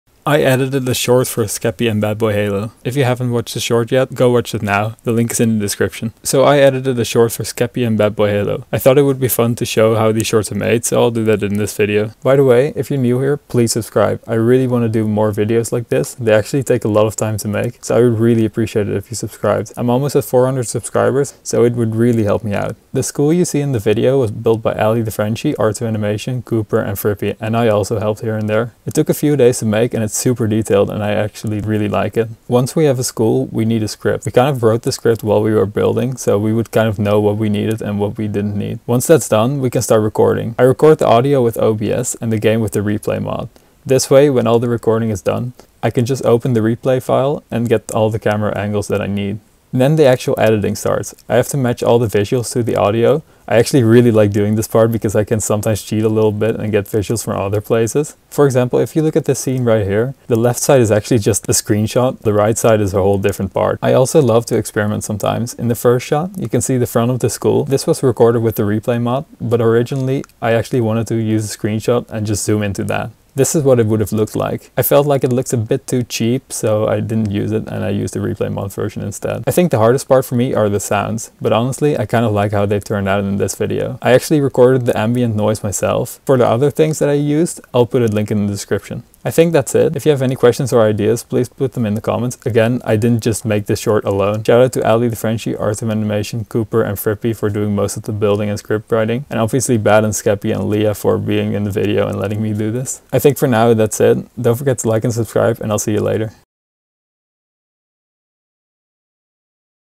I edited a Skeppy and BadBoyHalo short!
So I edited a short for Skeppy and BadBoyHalo. Here's a short VFX Breakdown or BTS (Behind The Scenes) of how that went.